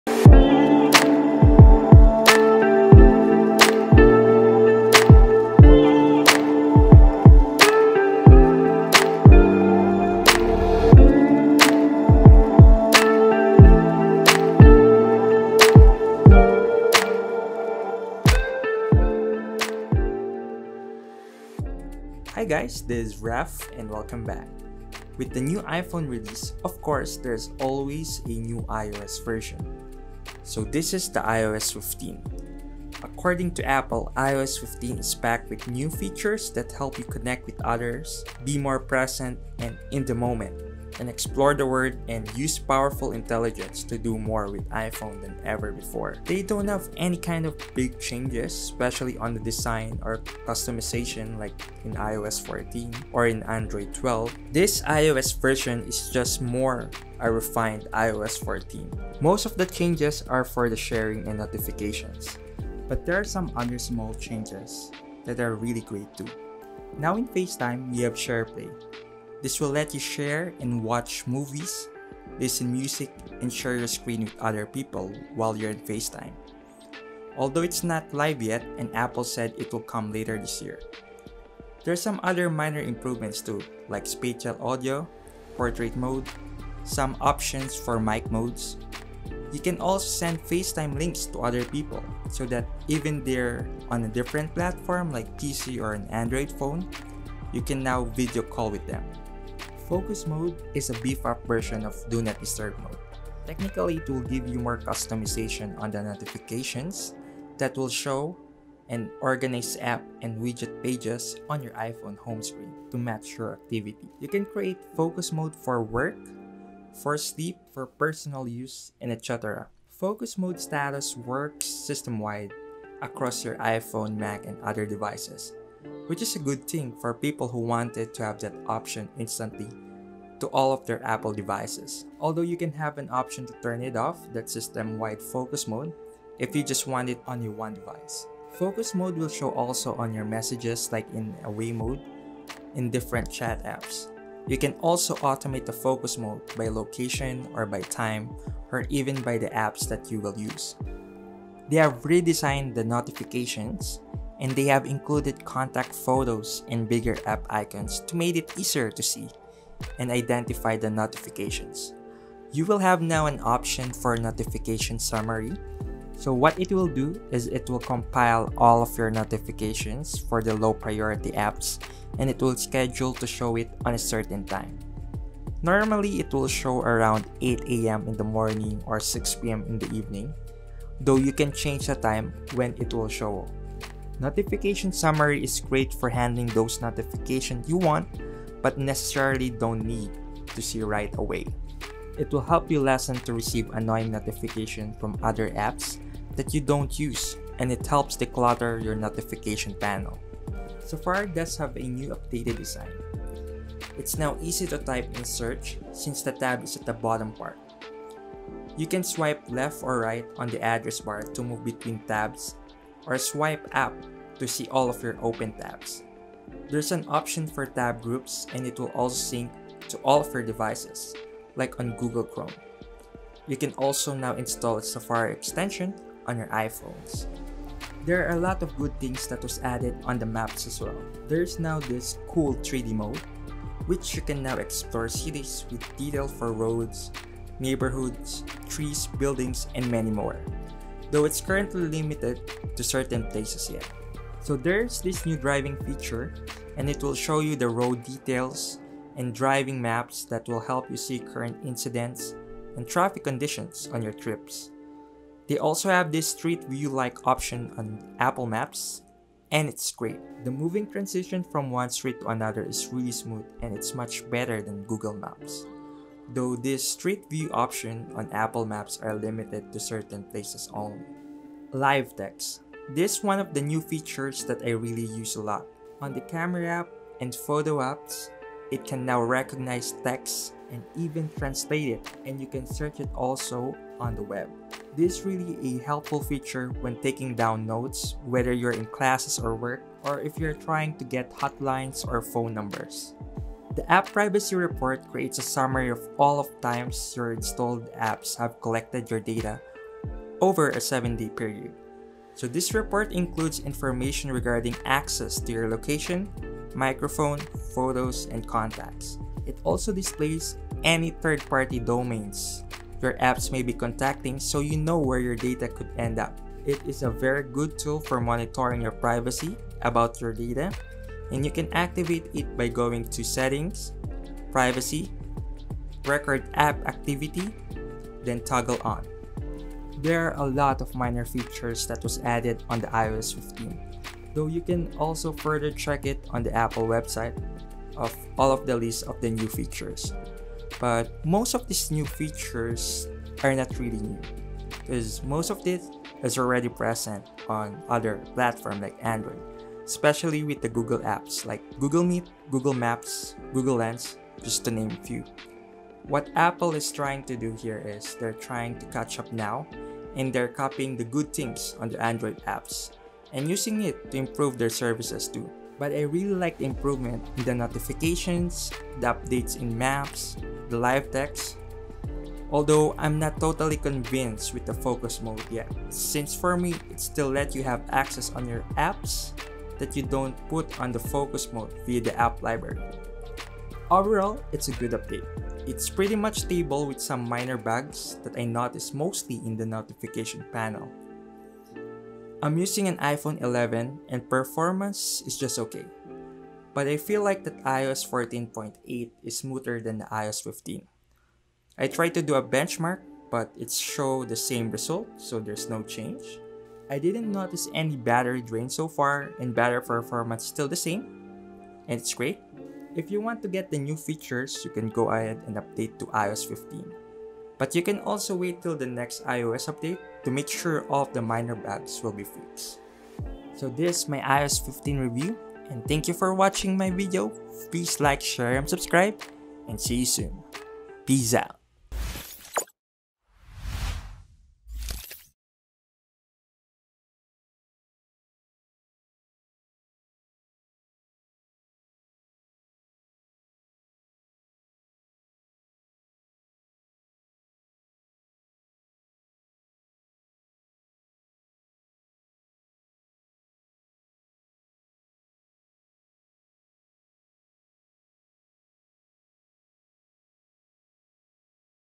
iOS 15 review
Here are my thoughts and review of the iOS 15.  I hope you like it all.

FAQ
Main phones:
iPhone 11 running iOS 15
Xiaomi Mi9t Pro running Android 10/Miui12

Main Rig:
Currently using Acer nitro 5 AN515-52
intel i5 8th gen with 12 Gb of ram

Main camera:
iPhone 11